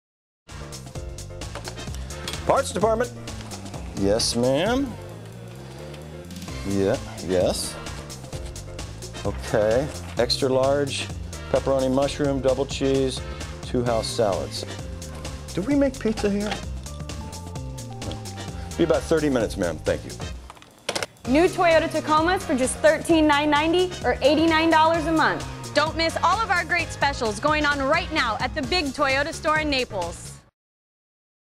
BOB GERMAIN WORKING IN THE PARTS DEPARTMENT - OR IS IT A PIZZA PLACE?
NICOLE GERMAIN IS DOING A GREAT JOB ON THE ADVERTISING AND BOB GERMAIN NEEDS TO KEEP BUSY!